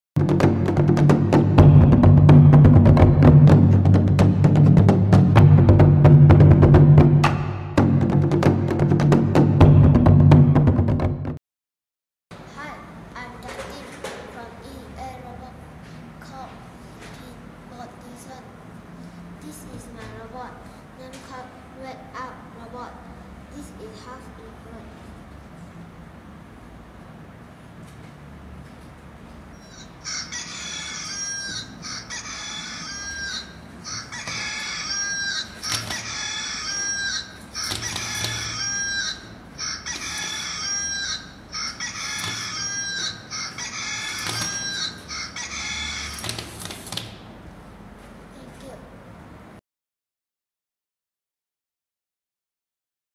[e@Robotclub] Justin's Wake Up Robot that made with ROBOTIS Dream kit
Justin Chin made a sleeping bed using the ROBOTIS Dream Kit, 1xServo Motor and he programmed this robot bed to detect the sound. The bed will shake when detect loud sound, thus the sleeper will wake up immediately.

The purpose of this innovative design project is to help a person to get up in the morning when hearing the sound of alarm clock.